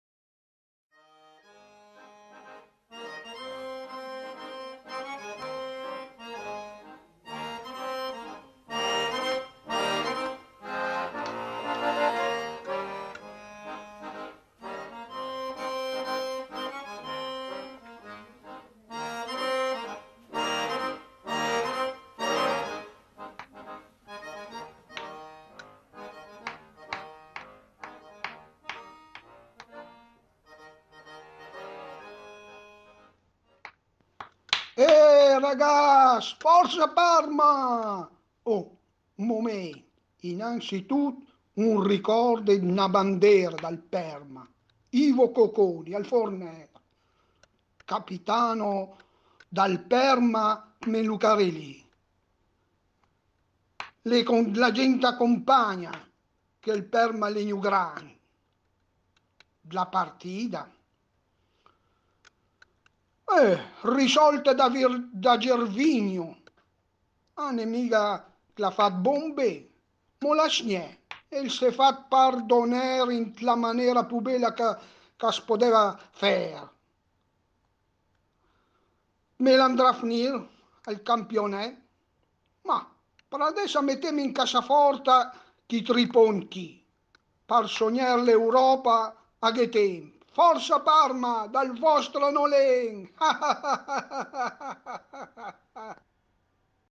Anolen su Sassuolo Parma 0 a 1
Anolén il popolare burattino di Ivano Ferrari commenta in dialetto parmigiano arios , le partite del Parma Calcio Voce , testo e burattino di Ivano Ferrari